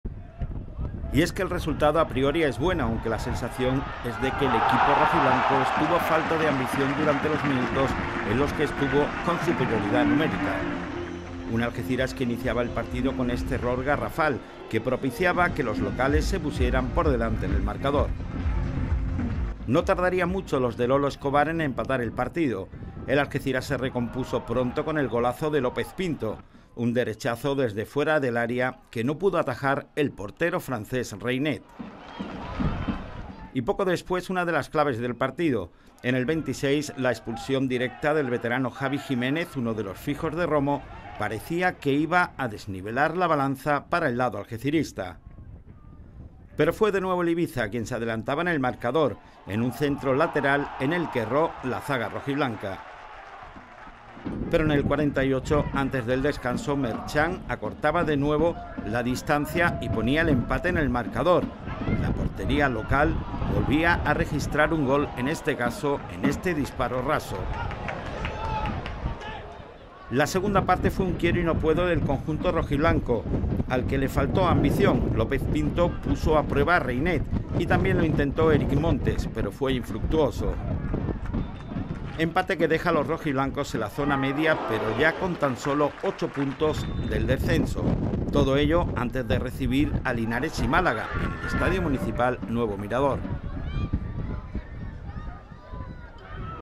El Algeciras empata en Ibiza con superioridad numérica durante 65 minutos
Y en deportes empate del Algeciras en su visita a Ibiza que se saldó con un resultado de 2 a 2 al término del partido. Por dos veces tuvo que remontar el equipo de Lolo Escobar que disfruto de casi 60 minutos con superioridad numérica pero que no pudo traducir en una victoria que en los minutos finales tuvo a su alcance.